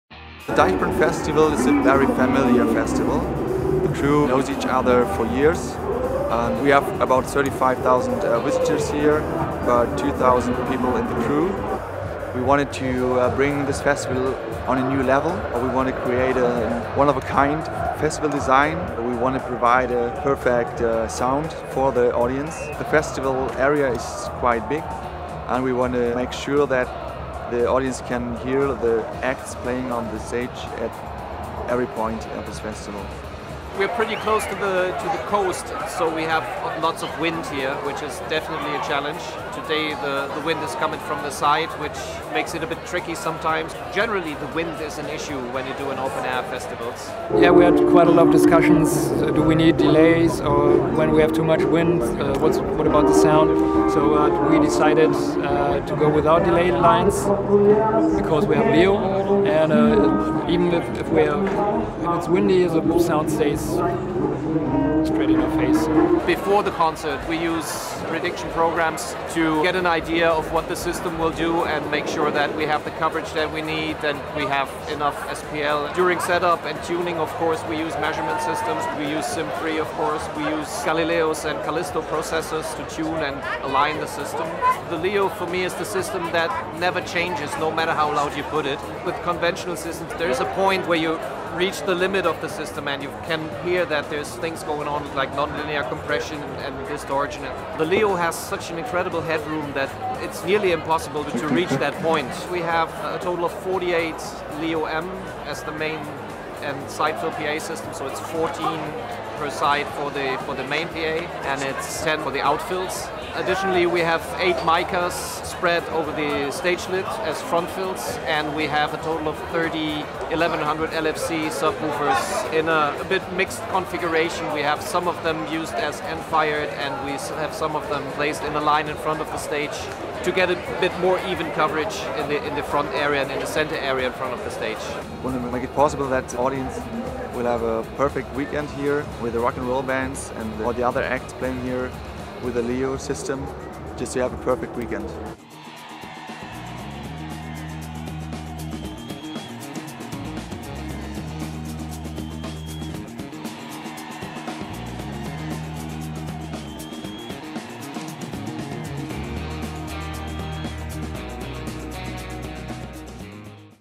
Meyer Sound LEO Line Array System at Germany's Deichbrand Festival
Headlined by acts including Bush, Die Toten Hosen, and Funeral for a Friend, the four-day 2013 Deichbrand Festival in Germany attracted around 35,000 people a day to an airfield near the North Sea. Rental house POOLgroup supplied a Meyer Sound LEO line array loudspeaker system to cover the entire 150-meter-long audience area for the festival's main stage.

Go behind the scenes with Deichbrand's production crew and learn what it takes to give fans the most enjoyable experience possible. Interviewees include: Felix Suwelack (Production Manager, Deichbrand Festival), Tim Humpe (Touring and Entertainment, POOLgroup), and Thomas Mundorf (Meyer Sound Tech Support).

"LEO is so much more powerful than anything else we have worked with. The sound stays focused and in your face, even in the most challenging conditions."

Tim Humpe
Head of Entertainment and Touring Production,
POOLgroup